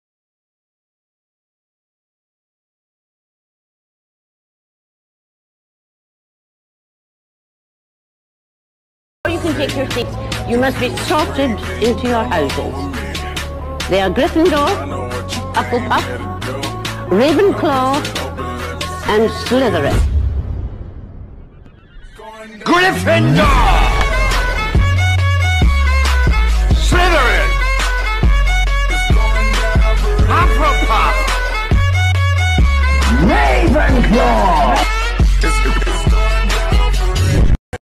Your houses meme | read desc
Should I have Harry Potter characters react to this?
Also I sorted me and my friends in our houses by our zodiac signs.